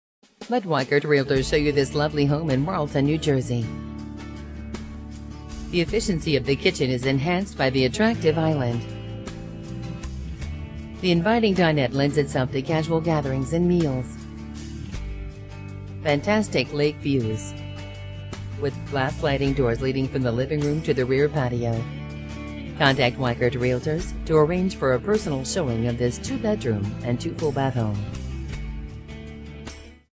Marlton, NJ Real Estate - Weichert
Condo/Townhome/Co-Op, Listed By Susan Martusus MLS Number: 5343095, 2 Bedrooms 2.00 Bathrooms